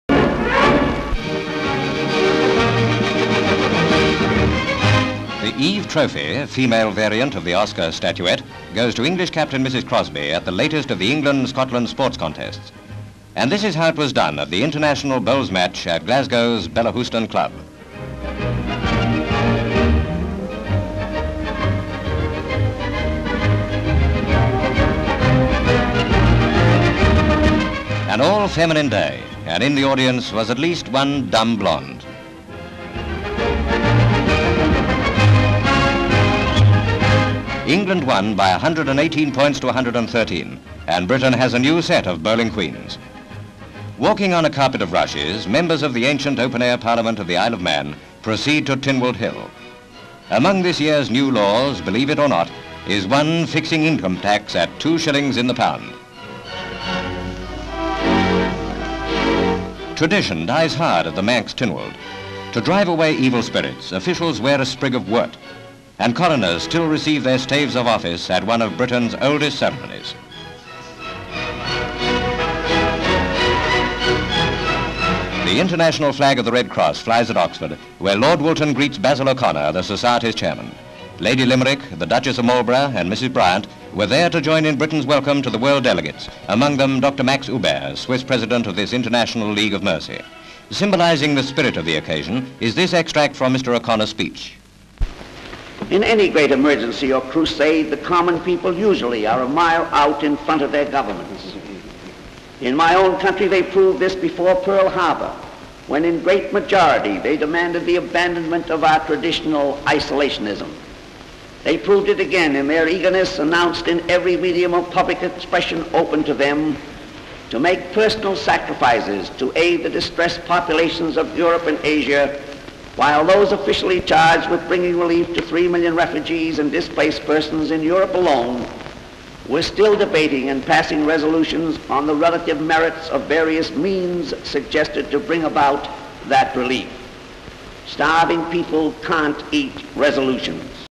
Seen In Camera (1946)
Item title reads - Seen in camera.

Various locations.

Bellahouston, Scotland. England versus Scotland women's bowls match. C/U of Mrs Crossbie, the English captain, receiving the Eve Trophy on behalf of the English team after they have won. C/U of Mrs Crossbie. Various shots of the match in progress. C/U of the scoreboard. C/U of Mrs Crossbie with the Scottish team captain.

Various shots as officials and dignitaries walk in procession to Tynwald Hill for the open air parliament on the Isle of Man.  A naval Guard of Honour presents arms. L/S of large crowds. C/U of a sprig of wort worn by official to drive away evil spirits. Various shots as the coroners receive their official staves of office in a ceremony.  L/S of the scene on the raised open air platform.

C/U of the flag of the Red Cross in Oxford. Lord Woolton shakes hands with Basil O'Connor, Chairman of the International Red Cross before the meeting. M/S of the Duchess of Marlborough, Lady Limerick and Mrs Bryant. Other delegates shake hands with Dr Max Huber. M/S of Mr O'Connor inside the hall making a speech. Various shots of delegates from different countries as he talks about people leading the way for Governments.
 FILM ID:1404.16